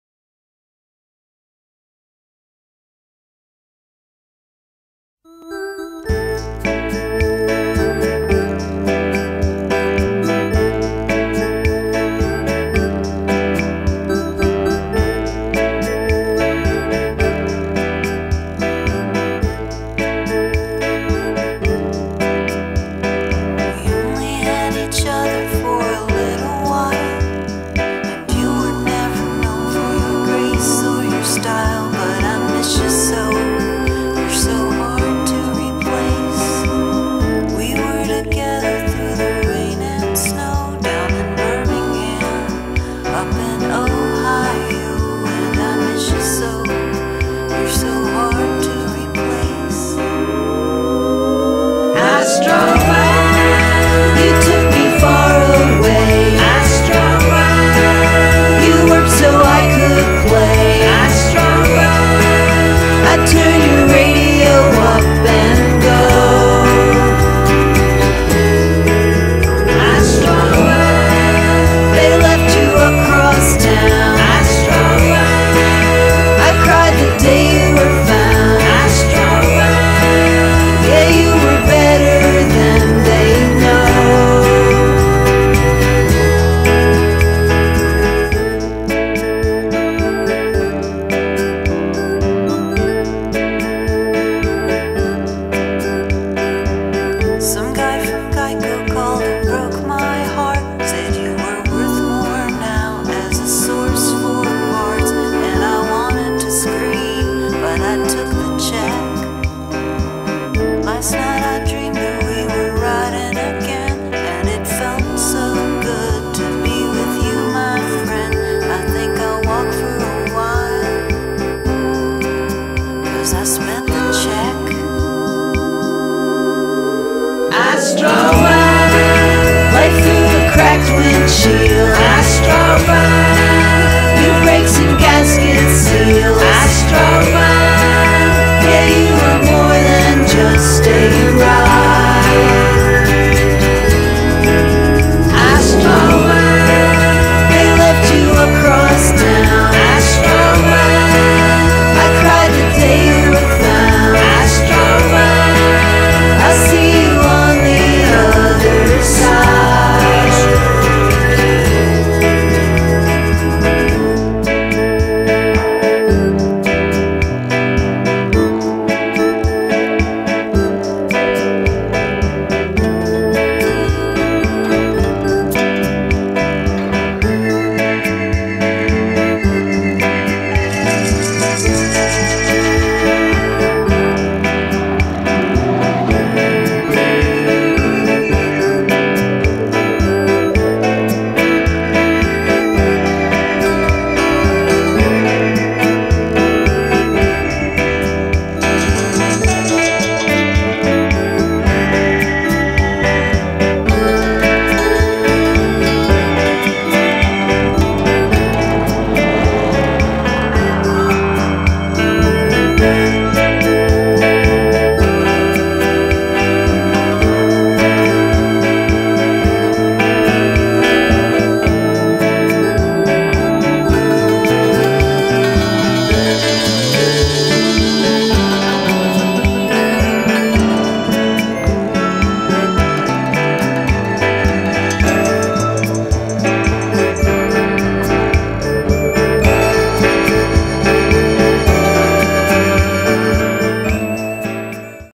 Astro Safari UK Forum Meets - Selection of Photographs
A selection of photographs of astrosafari.co.uk Astro/Safari vans taken at The Brooklands Museum, Wicksteed Park and Hatton Country World.